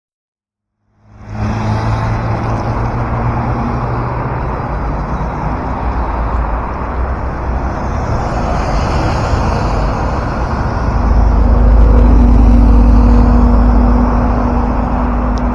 Химки. Автобус в старой московской окраске. 28 апреля 2024
Химки. Автобус в старой московской окраске. 28 апреля 2024 года.